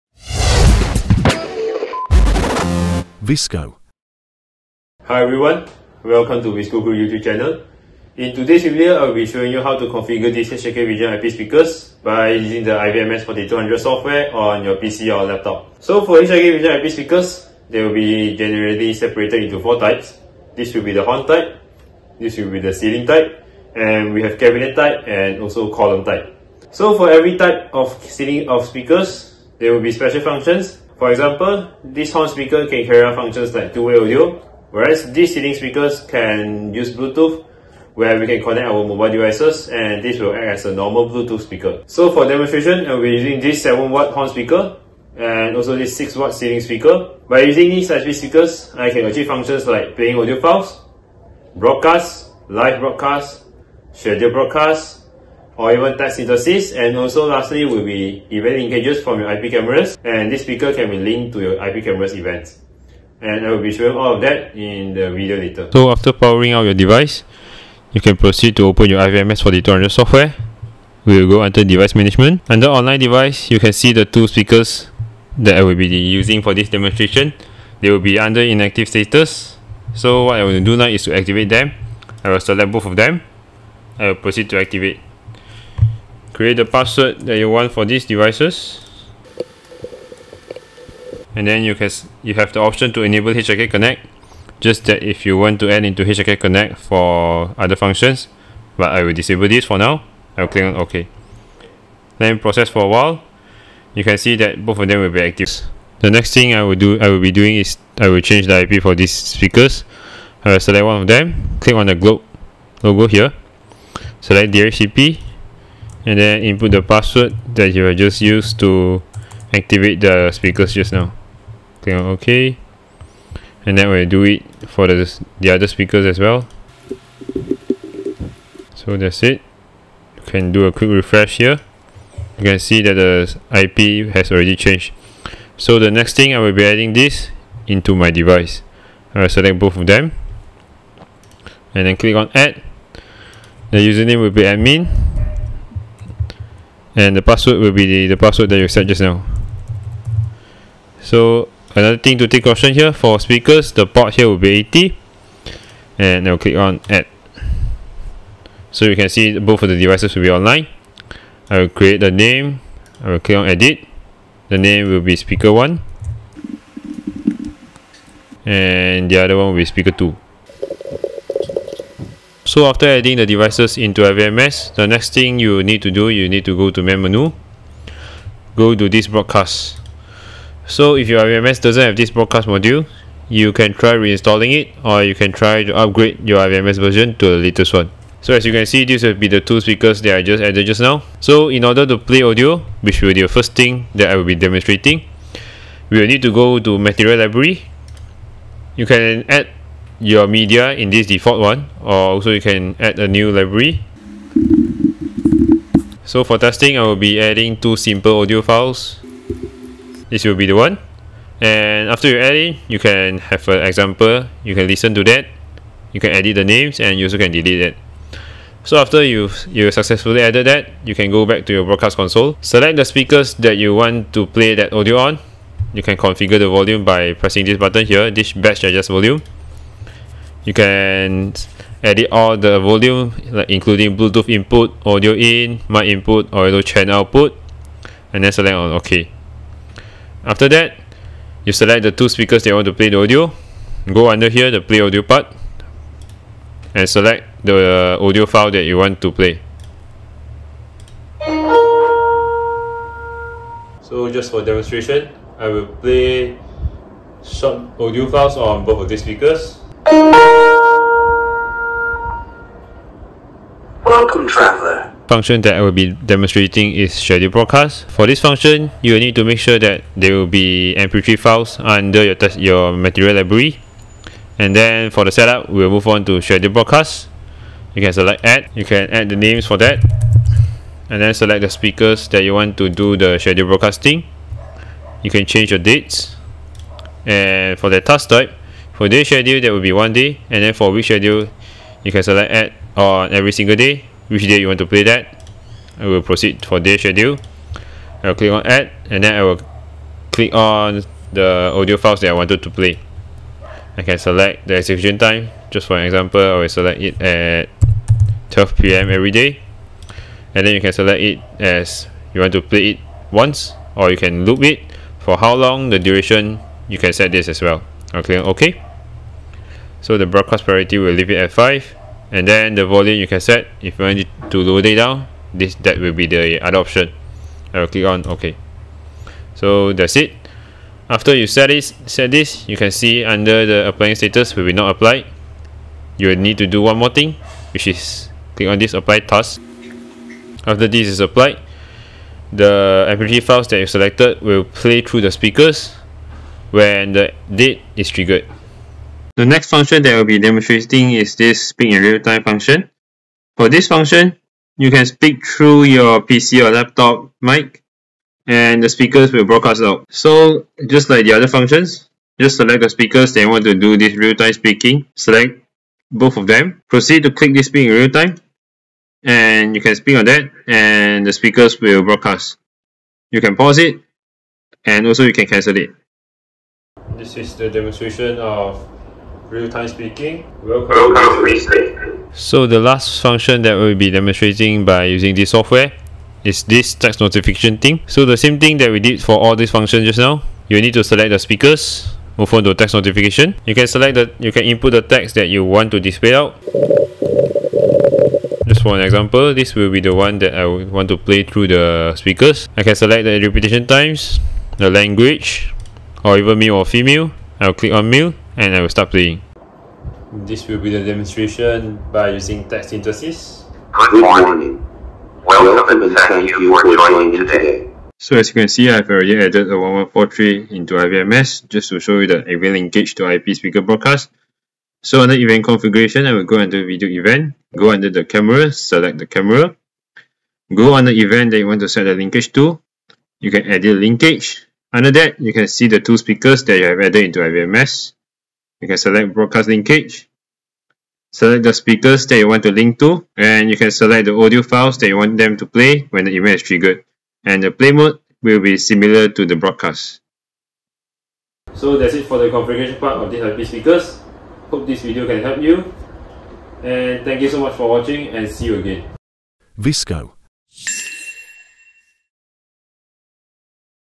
HOW TO : Configure Hikvision IP Speakers Using iVMS-4200
In this video, we will guide you through the process of configuring Hikvision IP Speakers using the iVMS-4200 software. Whether you're setting up a new public address system or integrating with an existing network, this tutorial will help you get your speakers up and running efficiently.